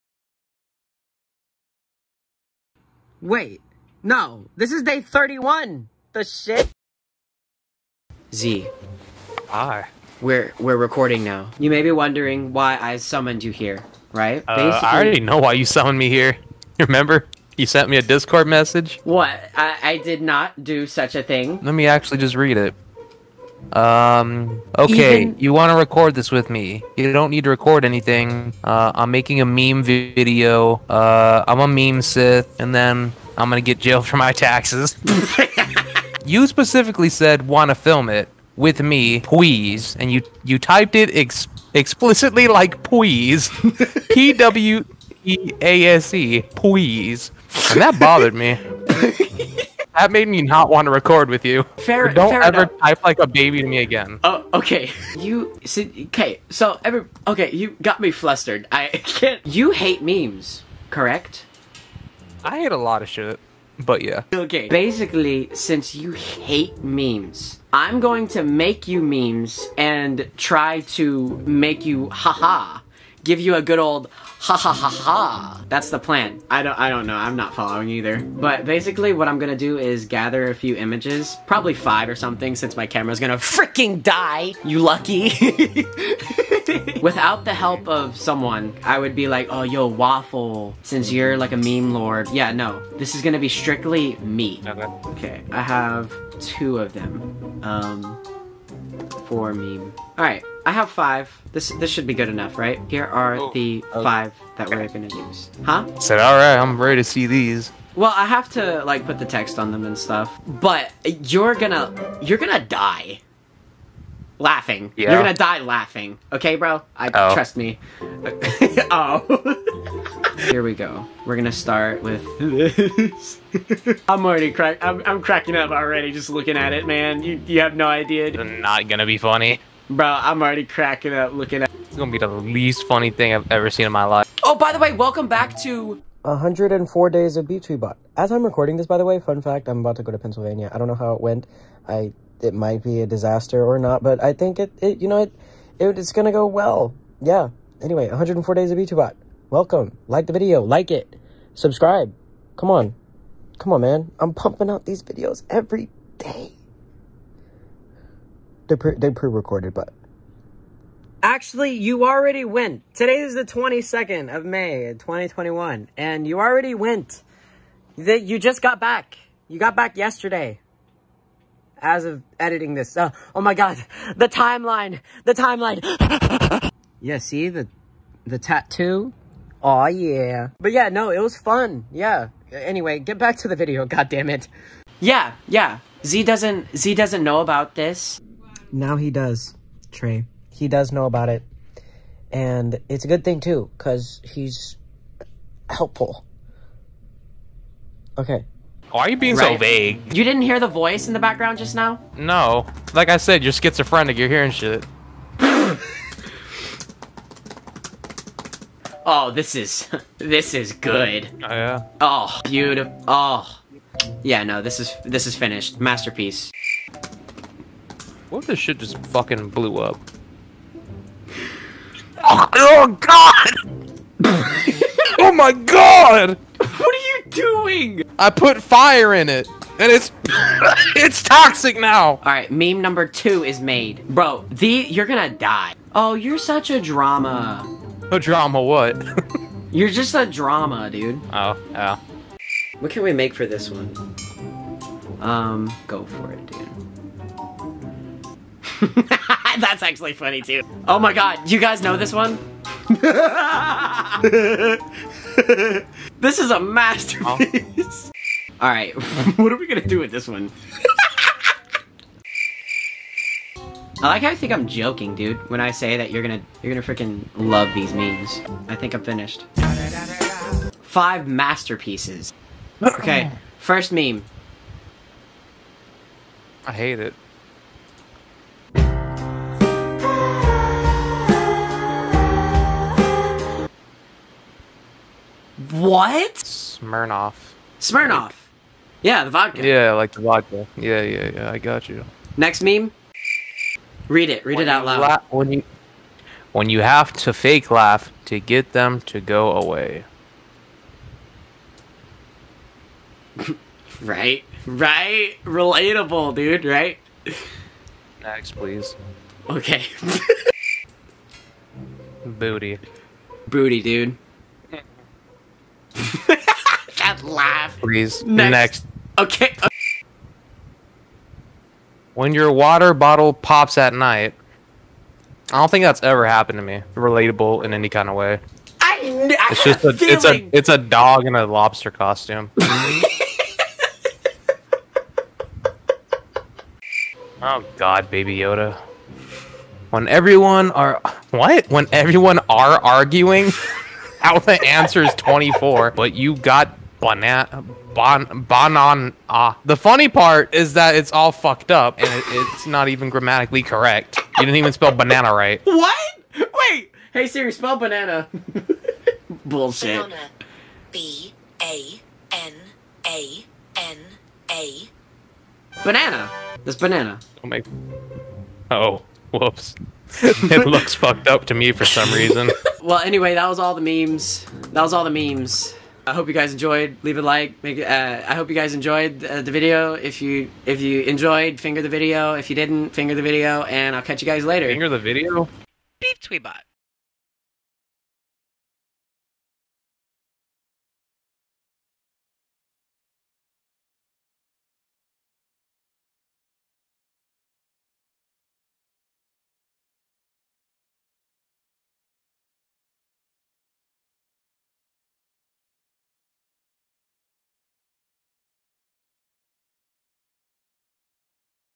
Trying My Hardest to Make Ze Laugh by Making Memes from Scratch
Day 31
Trey tries his best to make his friend Ze laugh by making some memes with his immensely deep knowledge on the world of memes.
Let's go for 5 LIKES! LOL!